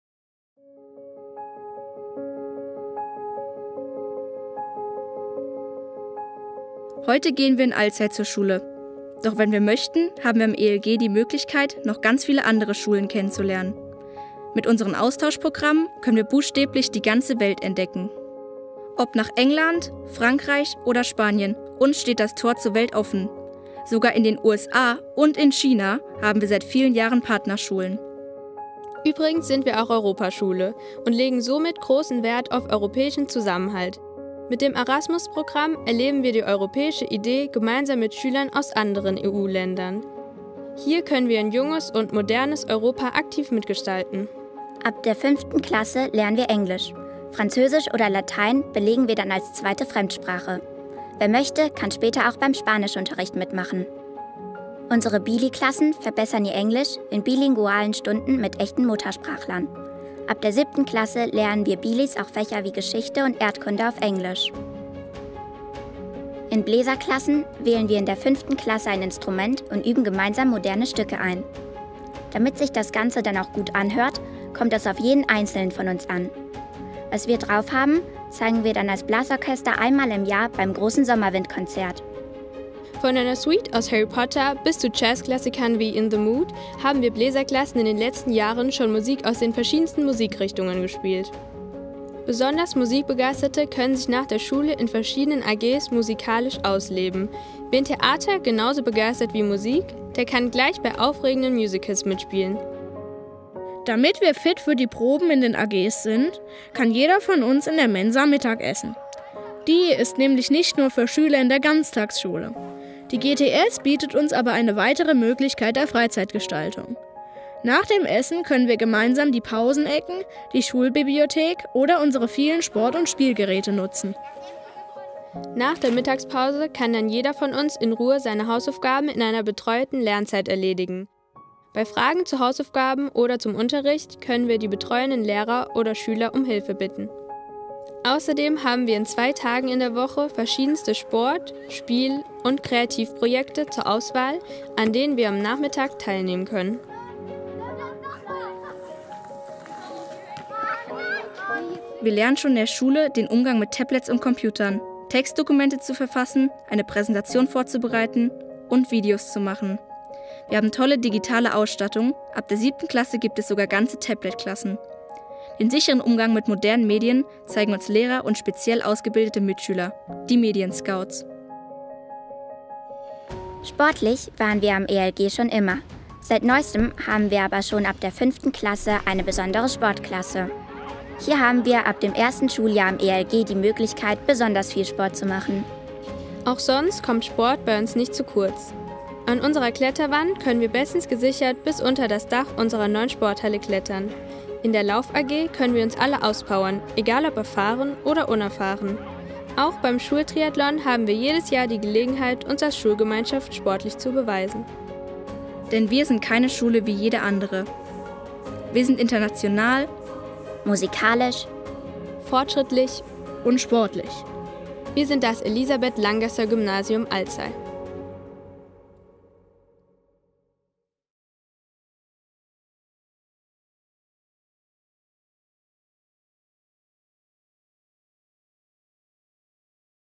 Das ELG im Kurzportrait // Elisabeth-Langgässer-Gymnasium Alzey
Willkommen am Elisabeth-Langgässer-Gymnasium!
Das ELG Alzey bietet vielfältige Möglichkeiten für alle Altersstufen.
Ob Austausche, Bläserklasse, Chor, Schulsanitätsdienst, bilingualer Unterricht, Sportklasse und mehr: Bei uns ist für jeden etwas dabei!
Zusätzlich sorgt das Ganztagsschulenprogramm bei jüngeren Schülern optional für eine altersgerechte Betreuung nach dem Schulalltag.

Weitere Informationen zu den Schwerpunkten unserer Schule folgen ab Mittwoch, dem 9.12. - Überzeuge dich selbst und schau dich um auf unserem Kanal und unserer Website!

Ein Film von Niv Adam, Luis Dreyer und Fabian Kästner.